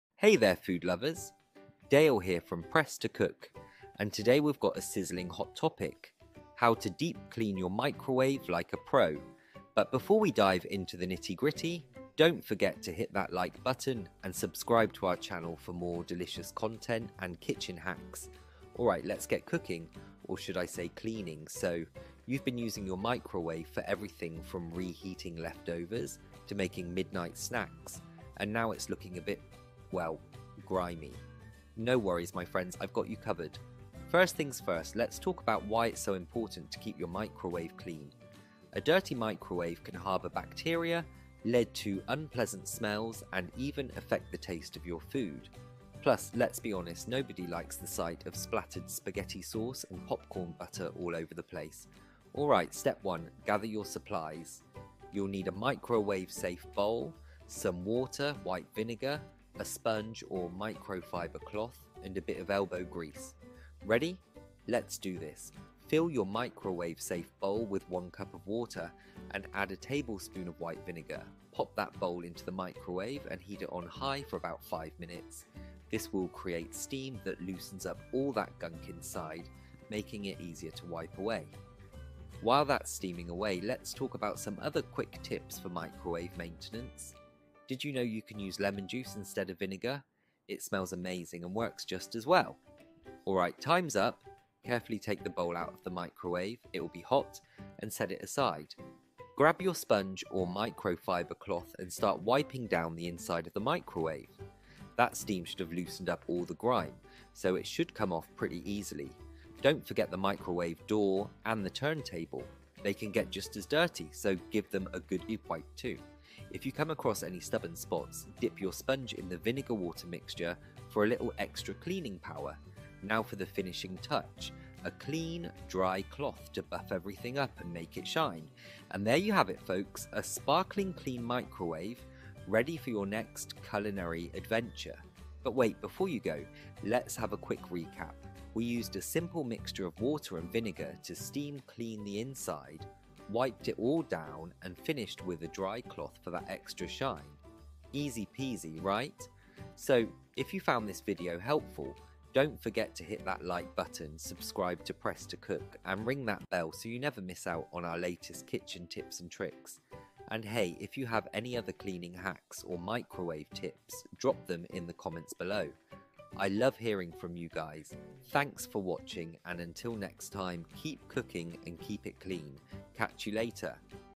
How to Deep Clean Your Microwave Like a Pro
In this video, Dale from Press To Cook shows you how to deep clean your microwave like a pro with simple steps and ingredients you probably already have at home! Say goodbye to stubborn grime and hello to a sparkling clean microwave that’s ready for your next culinary adventure.

Dale explains why it's important to keep your microwave clean, shares a few quick maintenance tips, and walks you through the entire cleaning process using a mixture of water and white vinegar (or lemon juice for a fresh scent!). With his fun and upbeat style, you'll find this chore not only easy but also enjoyable.

Got your own cleaning hacks or microwave tips? Share them in the comments below—we love hearing from you!